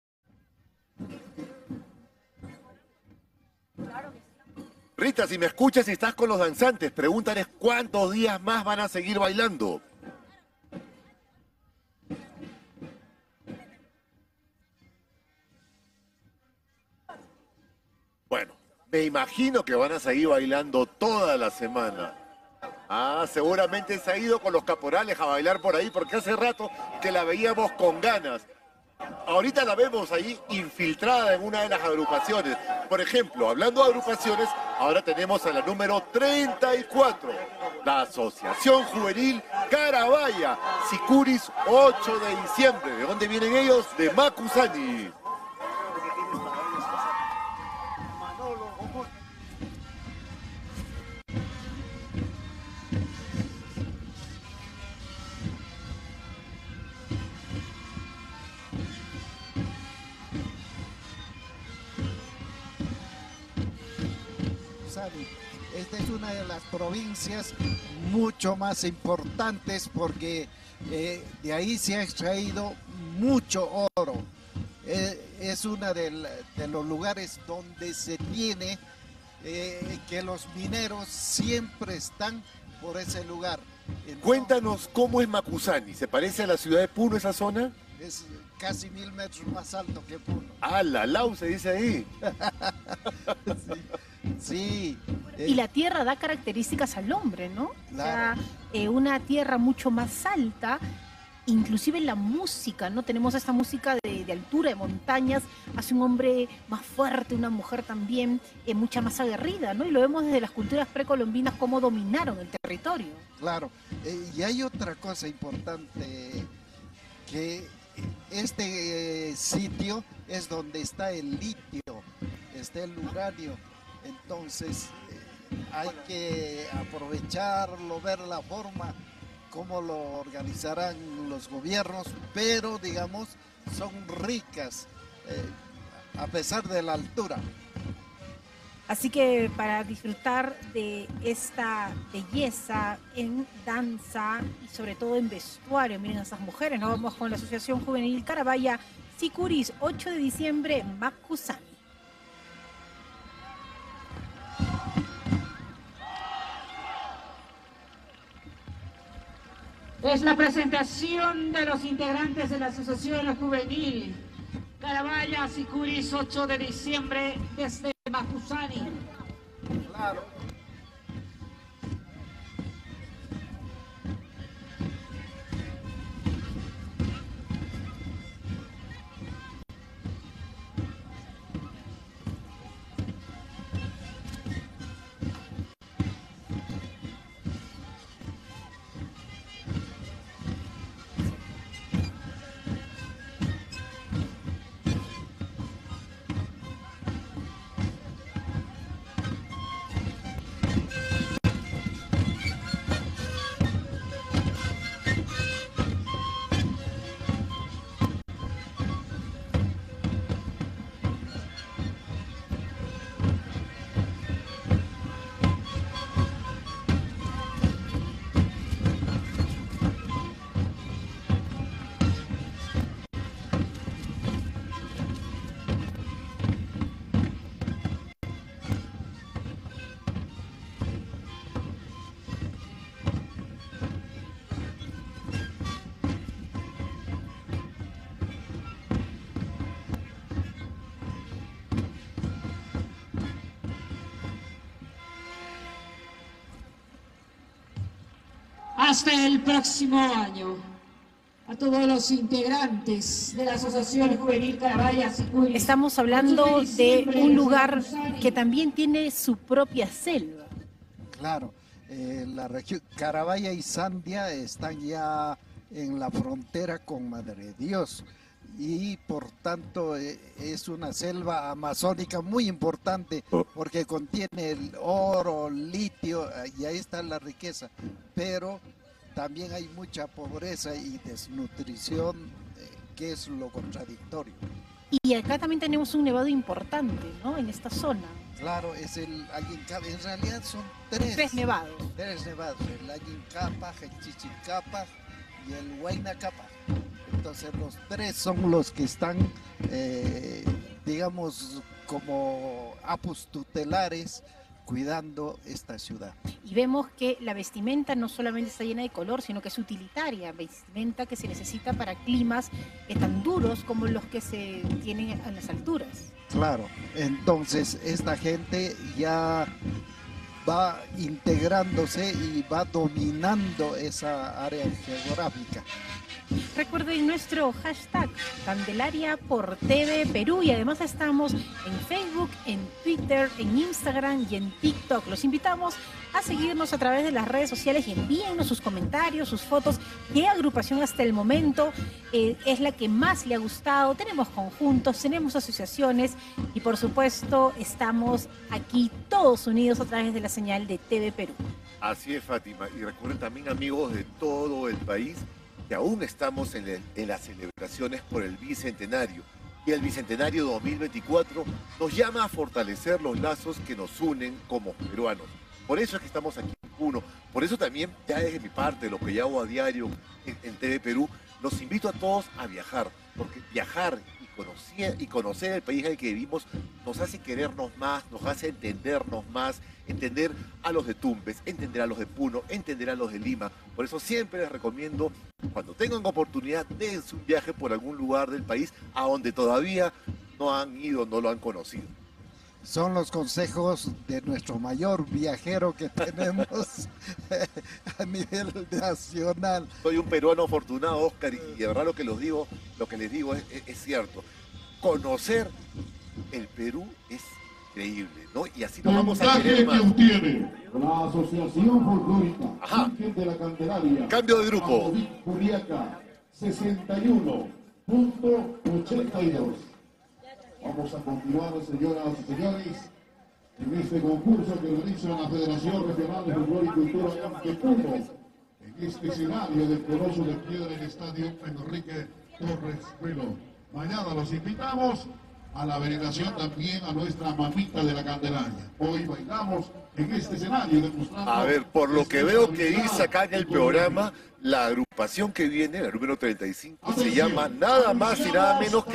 Sikuris 8 de Diciembre Macusani, Festividad de la Virgen de la Candelaria 2024 ,13va parte
Festividad de la Virgen de la Candelaria 2024 ,Presentación de los Sikuris 8 de Diciembre  Macusani  en el Estadio Enrique Torres Belon de Puno.